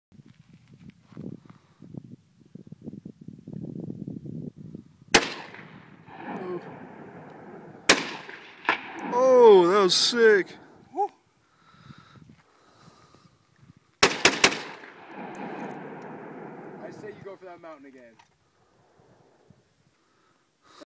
Mini 14 5.56 tracers
Ruger Mini-14 shooting american eagle 55gr 5.56x45mm tracer ammo. 5 shots